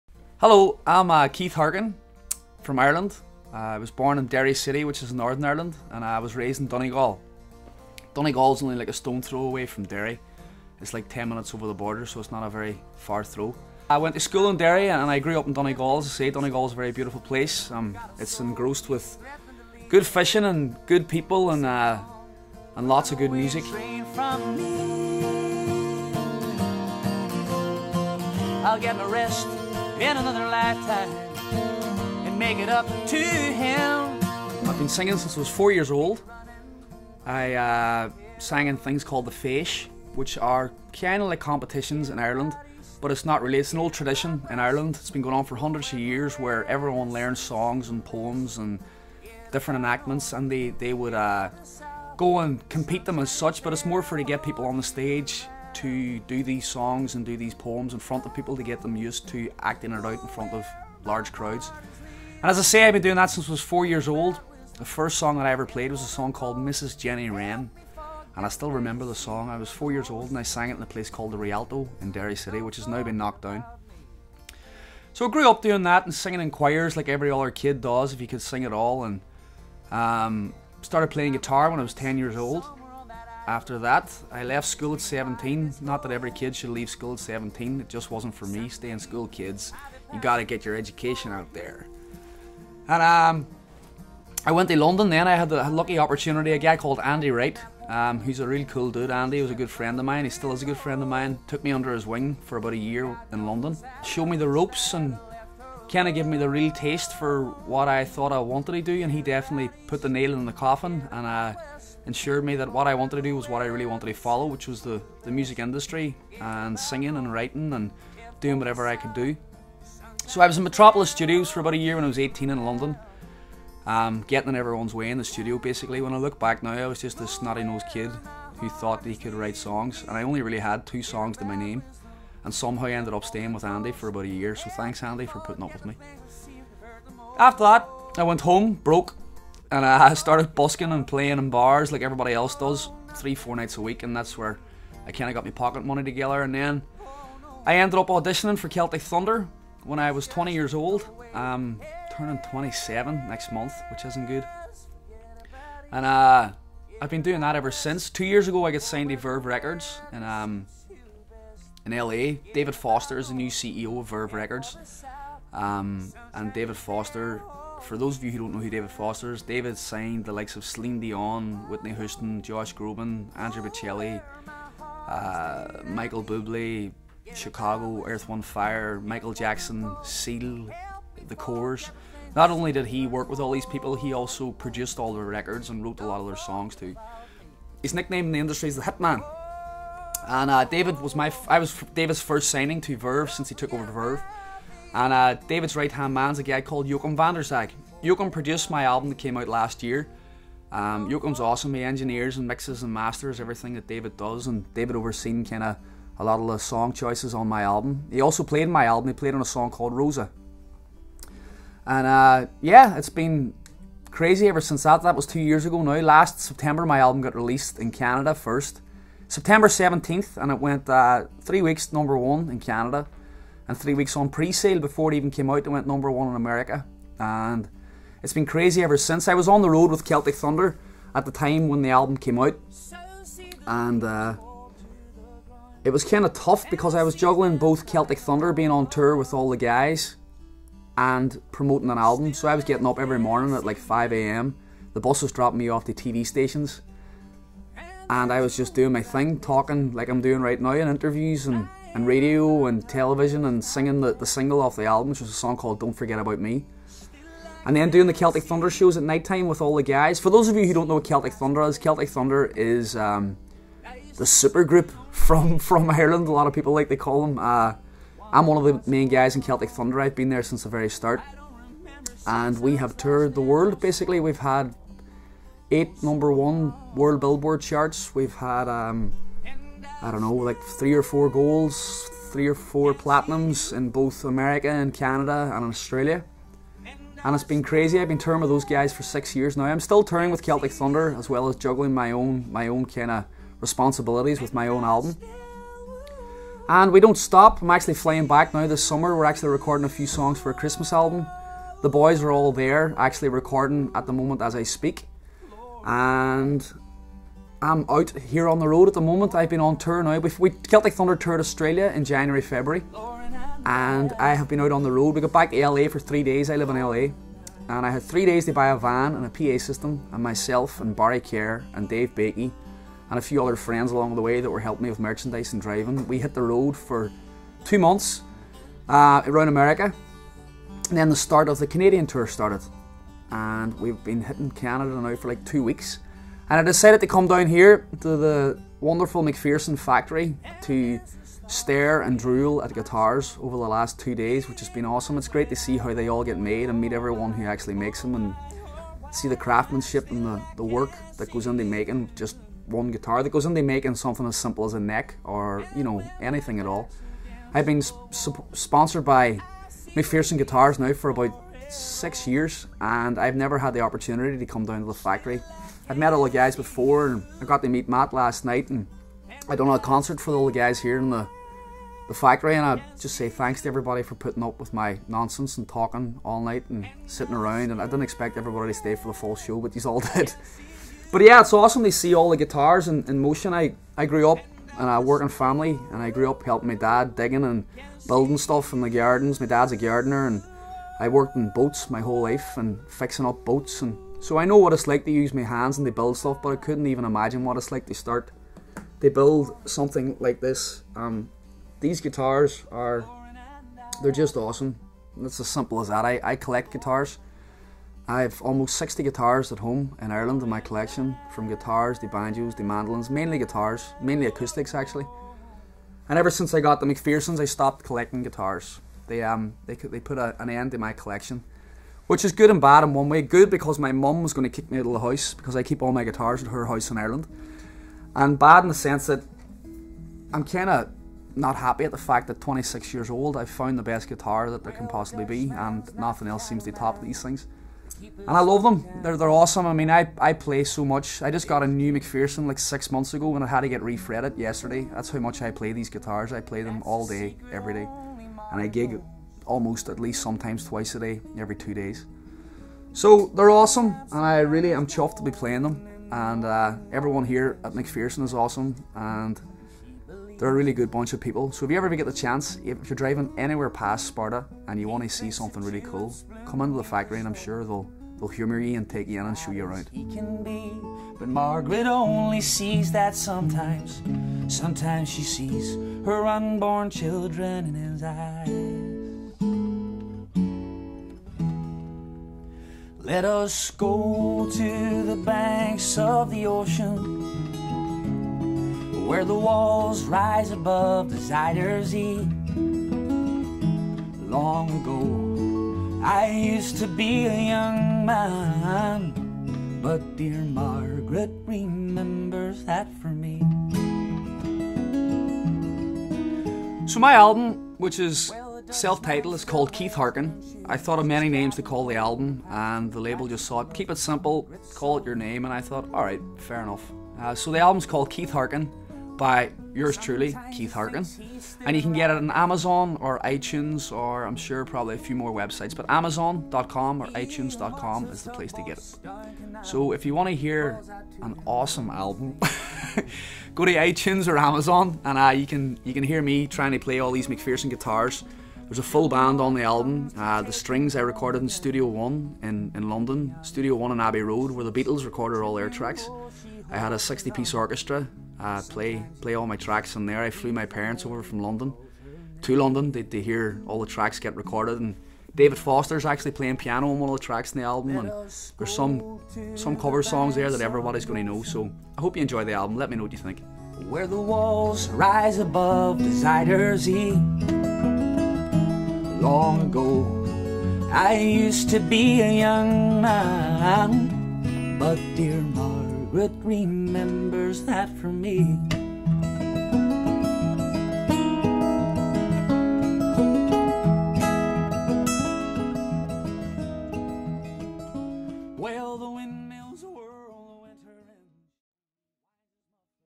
Artist Interview - July 2013 - Keith Harkin
Get to know McPherson Artist Keith Harkin in July 2013's artist of the month interview. For more clips like this & monthly Acoustips, become our friend